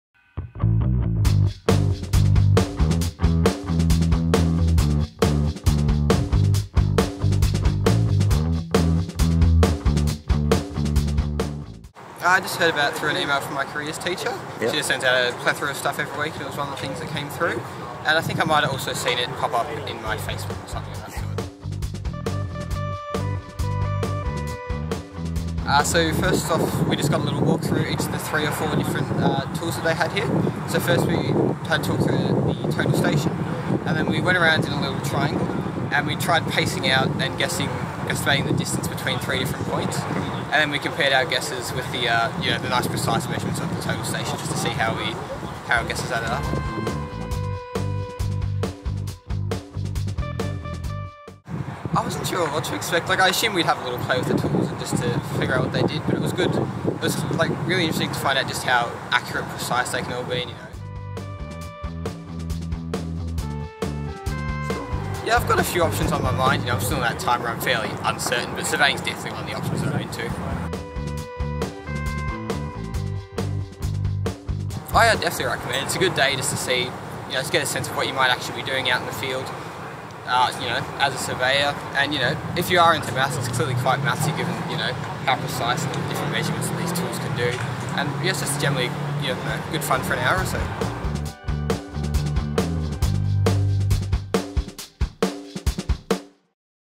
Speaking with a Student at the RMIT Experience Surveying Day
The RMIT Experience Surveying Day is back and off and running! Last week, Spencer was one of the students who had the chance to experiment with cutting edge technology, challenge themselves with surveying problems and learn a little more about what surveying can be like as a profession to work in. Hear Spencer's thoughts in the video, then book your place on one of the upcoming monthly events via this